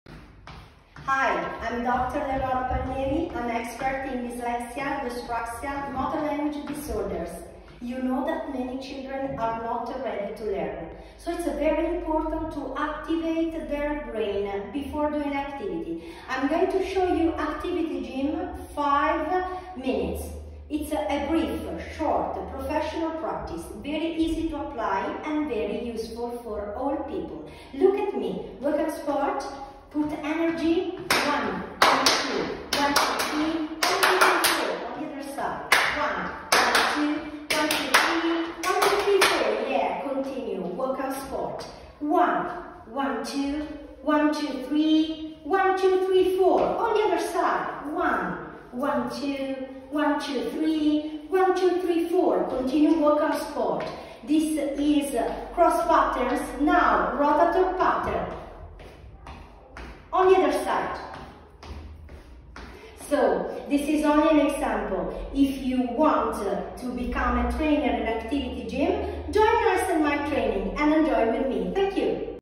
Dr. Palmieri April 2021 Activity Gym Rapid Neuro-Activation - Italy
Dr. Palmieri models one of the sequences from Activity Gym.

Activity Gym is a specific progression of rapid activation exercises created by Professor Piero Crispiani and Dr. Eleanora Palmieri using motor activities to enliven motor-cognition within 5 -10 minutes.

Activity Gym exercises are done quickly and intensively to engage motor-cognition for better sensory integration of the neural networks in the brain. Employing ipsilateral and contralateral movement sequences, Professor Crispiani and Dr. Palmieri engage cognitive-motor brain function to enhance neuroactivation in dyslexia, dyspraxia, ADHD, and neurodevelopmental disorders.

Activity Gym can be implemented during activities of daily living, early in the morning before going to school or work, before homework, before classroom lessons, and anytime the brain and body would benefit from alerting engagement. Activity Gym is useful for all ages and can be done by teachers, clinicians, therapists, and parents.